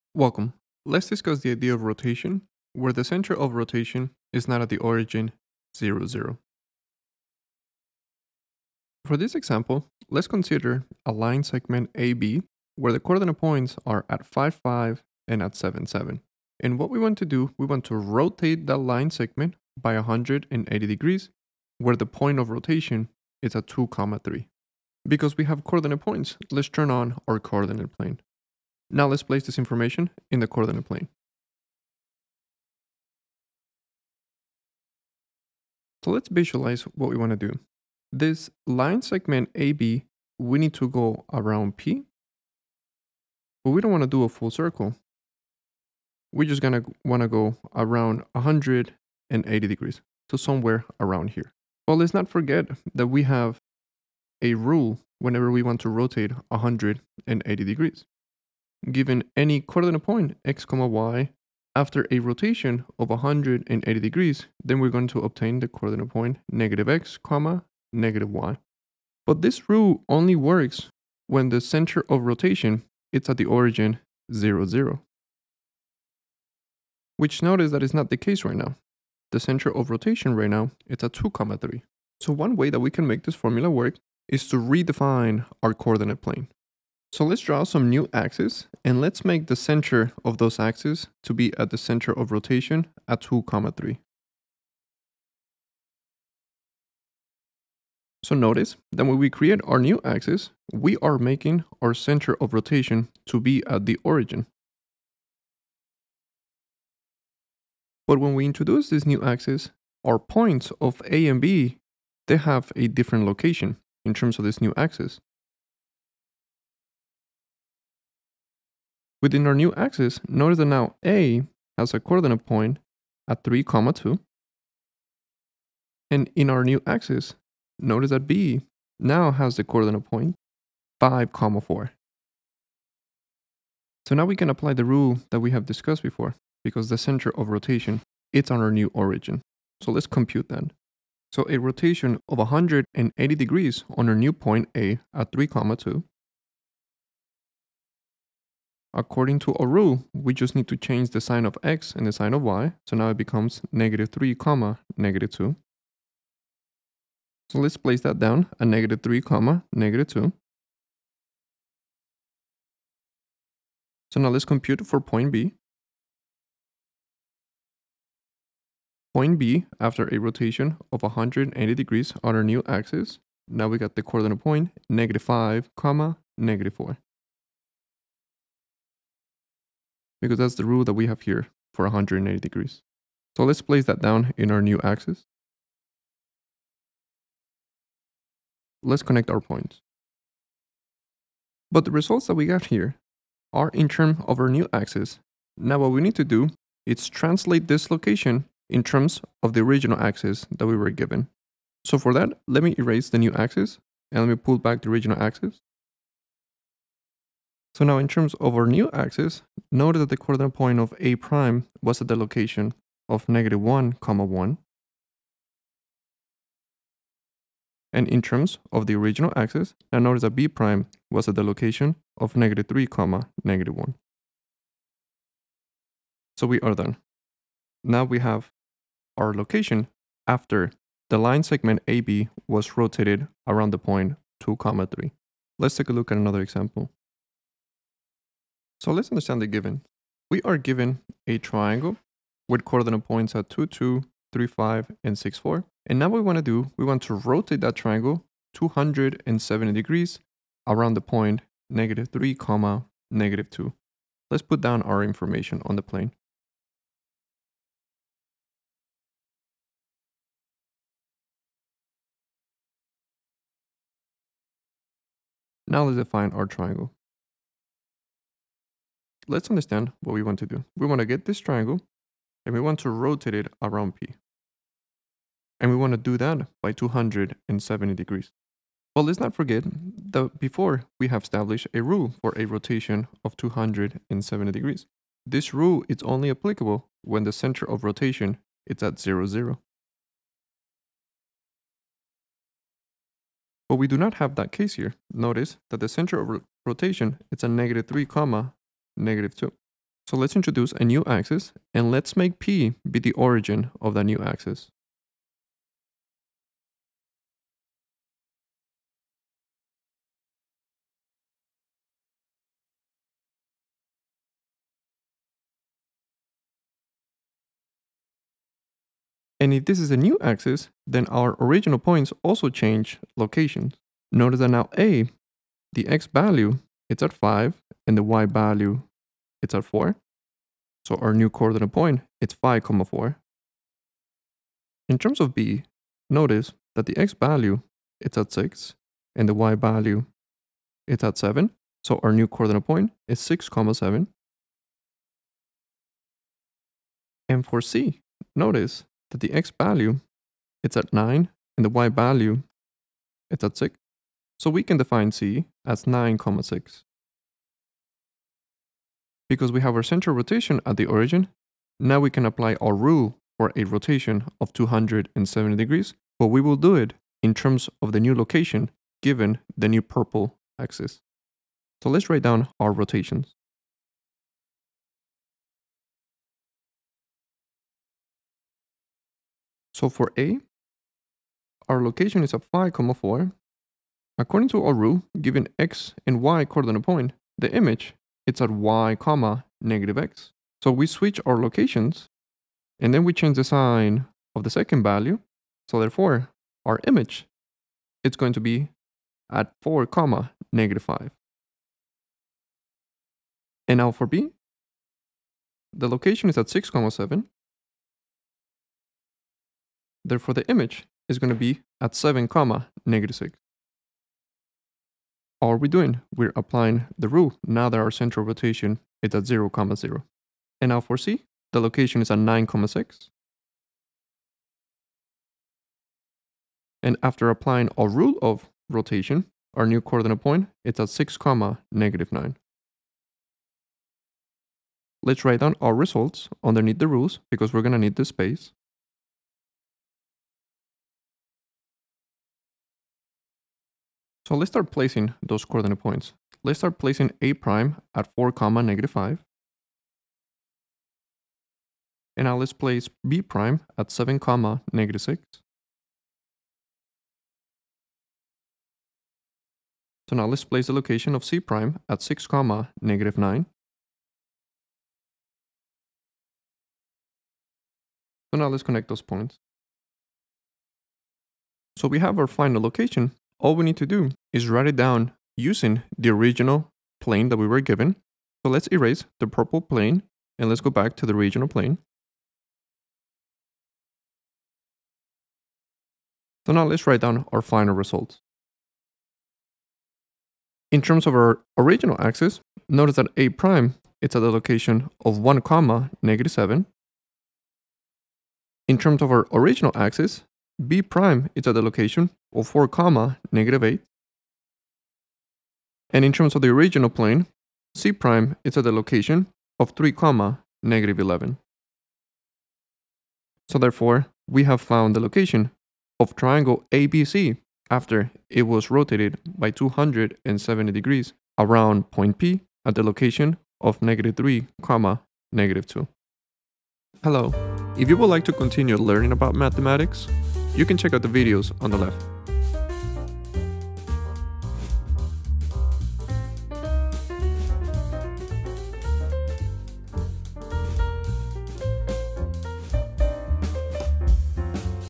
Rotation in the Coordinate Plane - Not Centered at Origin (Method: Moving the Plane)
Rotation in the coordinate plane - Not centered at origin is a lesson that will teach you how to perform a rotation in the coordinate plane when the center of rotation is not at the origin. Previously we developed rules for when we rotate an object about the origin, can we still use this rules? At the end of the lesson, you should be able to rotate an object about when the center of rotation is not the origin (0,0).

⏰Timestamps⏰
0:00 Introduction
1:00 Remembering rule of rotation of 180 degrees
1:30 Introducing new plane
2:22 Rotation of point in new plane
3:27 Defining location in original plane
3:55 Rotation of object not centered at origin (Example 2)